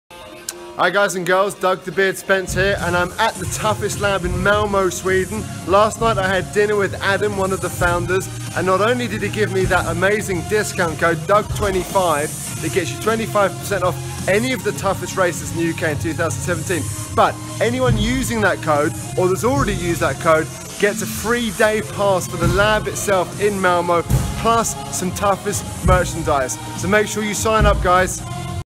Toughest DOUG25
Check this out, DOUG25 not only gets you 25% of all Toughest UK races in 2017 but those using it (or who have already used it) will get a free day pass to the Lab itself plus some Toughest kit!
I've just got back from 2 days at the Lab and it is absolutely epic, lots of videos to come!
Bookings for London South close this weekend so don't hang about!
Toughest
The Confidence Centre AKA The Backyard Jam